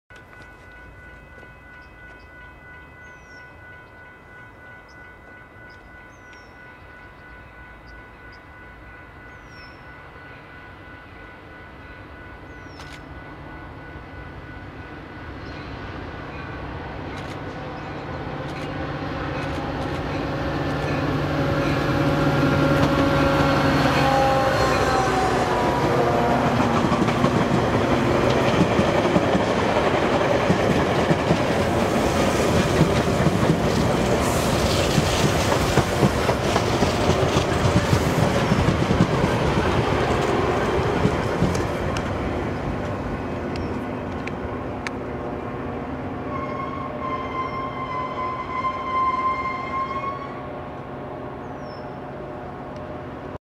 Metra 151 at Crystal Lake/Pingree Road
A late running Sunday Metra train pulls into Pingree Road station in Crystal Lake, IL - 7/5/09